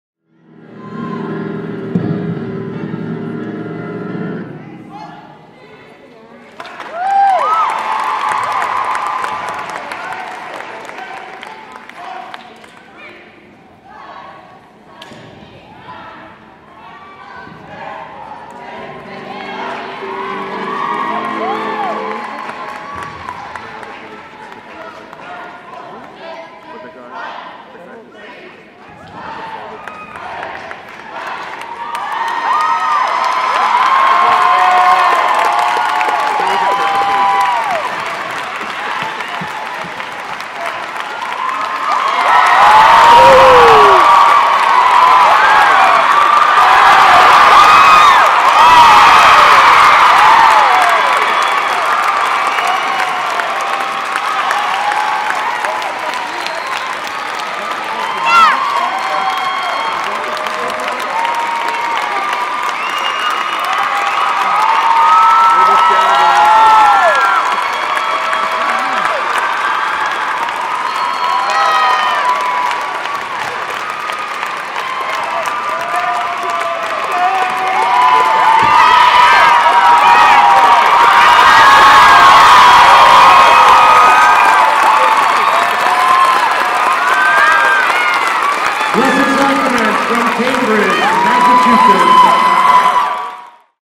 Blessed Sacrament 2018: AMAZING Performance Without Music
Blessed Sacrament 2018 Winter Guard stuns the crowd when they continue to perform their show once the music cuts out at the WGI East Power Regional.